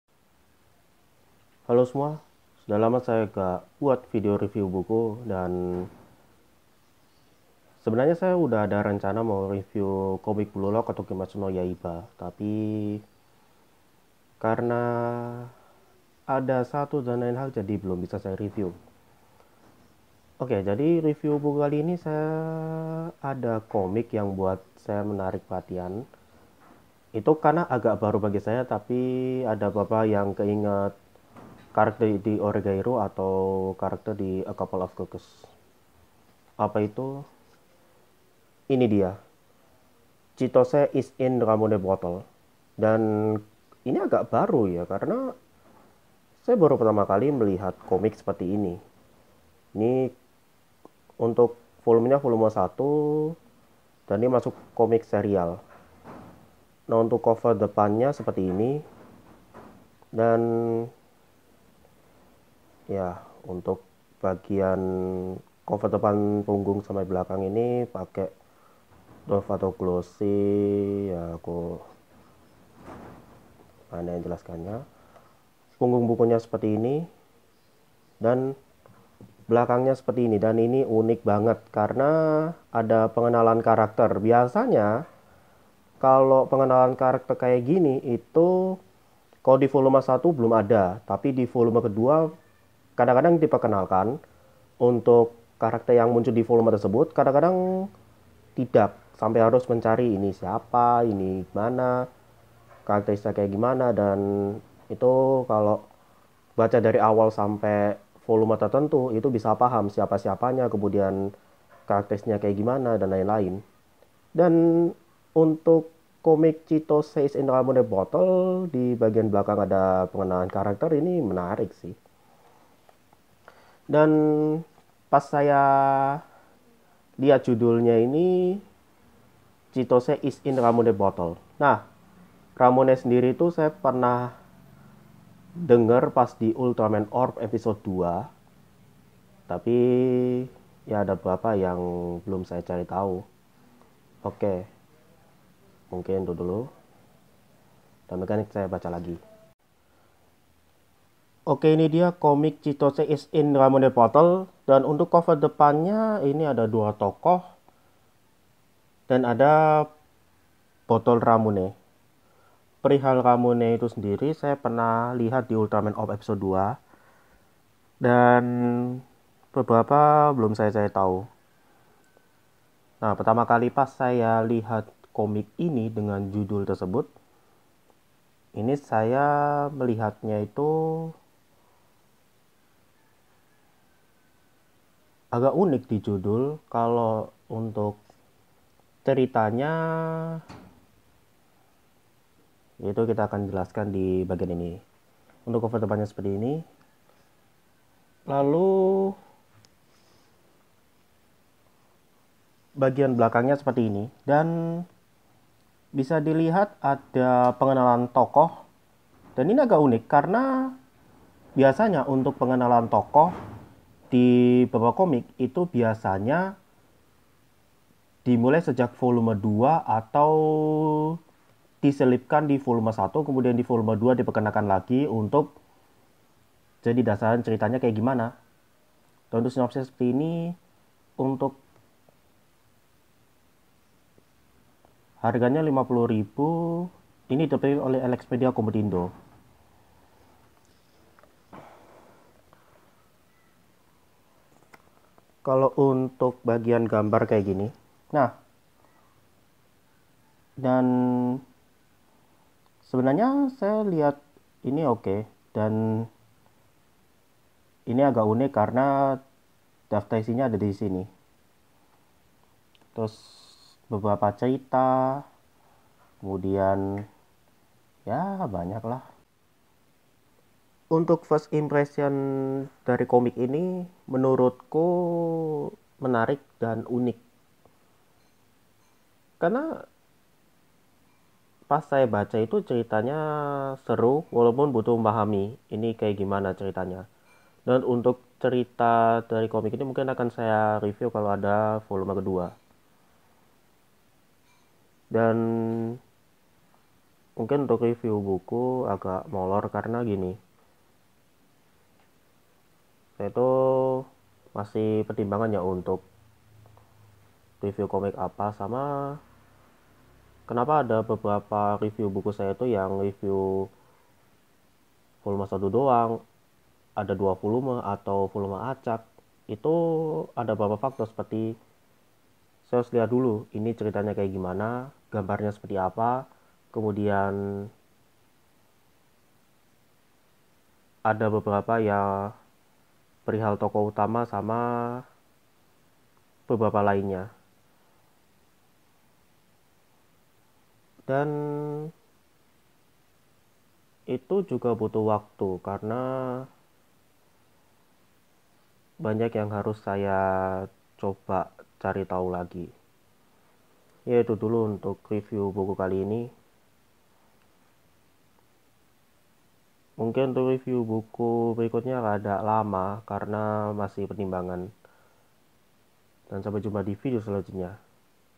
First Impression Saya untuk Komik Chitose is in Ramune Bottle
Chitose is in Ramune Bottle merupakan komik adaptasi dari buku novel ringan dengan judul yang sama. Review Buku kali ini adalah first impression saya untuk komik ini di Volume 1